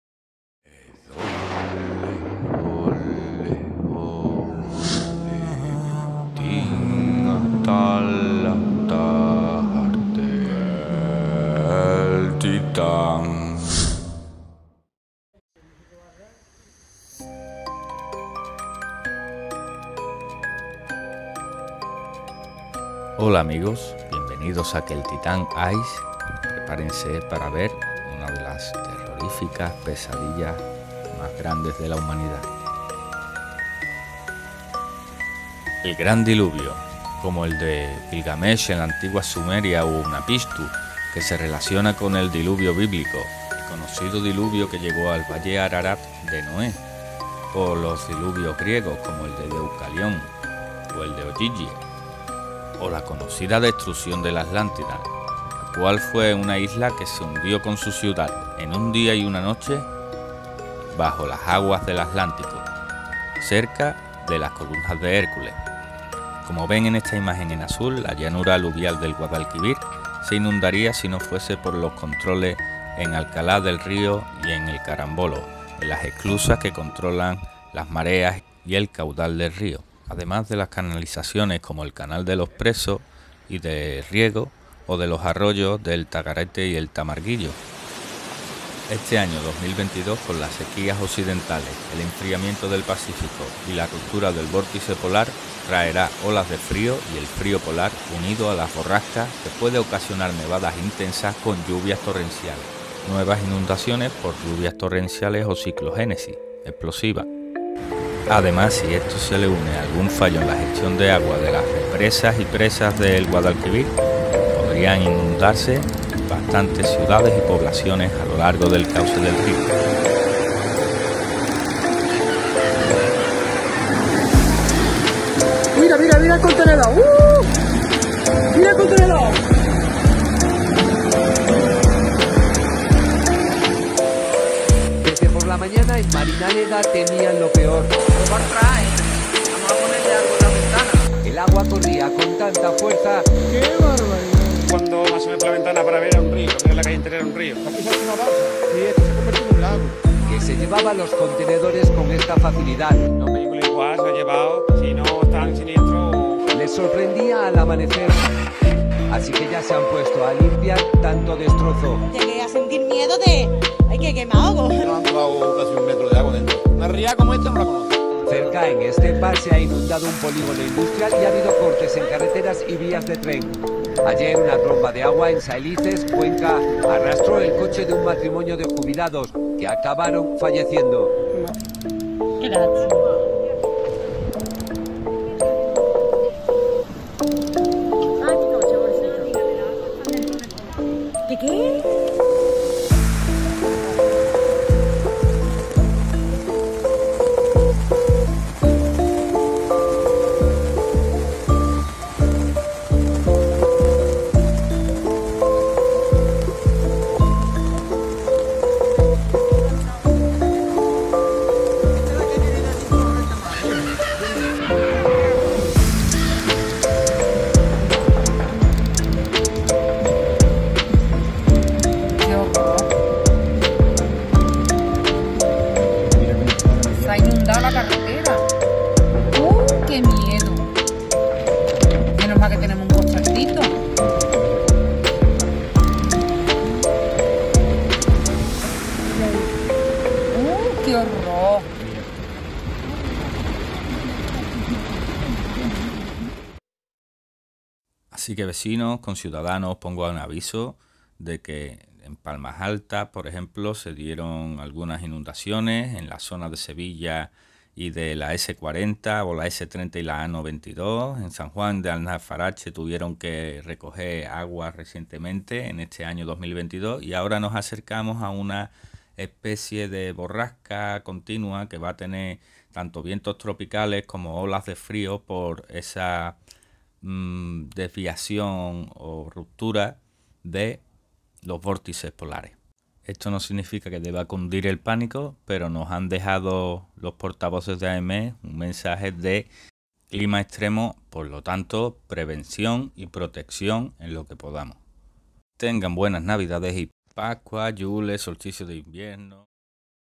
El Gran Diluvio Universal en Sevilla Keltitan ais
El Gran Diluvio... muchas leyendas alrededor de todas las culturas pies no es un hecho aislado. Sobrecoge ver imágenes reales y de actualidad. El clima nos encanta pero también nos impacta.

Pueden encontrar mas en la Web Keltitan ais

Hoy traigo unas imágenes de inundaciones en Sevilla y sus municipios. Que se han dado en los últimos años en lluvias torrenciales un poco inesperadas pero que no nos coja desprevenidos.

Les presento un video impactante de las últimas inundaciones en la provincia de Sevilla entre 2021 y 2022. Estas imágenes han sido tomadas de las redes y utilizadas libremente para informar y prevenir de una forma entretenida y sobre todo ilustrativa. Ante el riesgo real de inundación para que la población pueda reflexionar y plantar unos protocolos de actuación personal en su familia y su zona. Han de tomarse unas contramedidas de actuación en caso de riesgos naturales y conocer el terreno para ponerse a salvo el tiempo necesario.

Músico: SoundGalleryByDmitryTaras

Y

Músico: SoulProdMusic